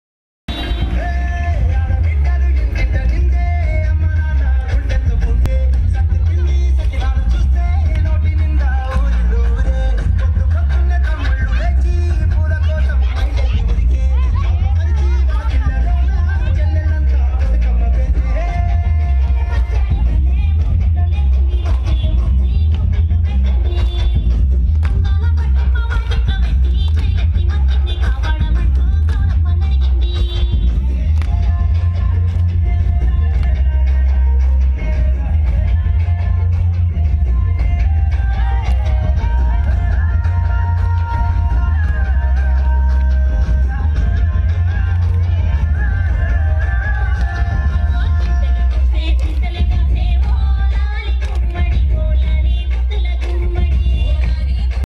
23 October 2023
సద్దుల బతుకమ్మ @ సుజాత నగర్
Last day of Bathukamma
Saddula Bathukamma celebrations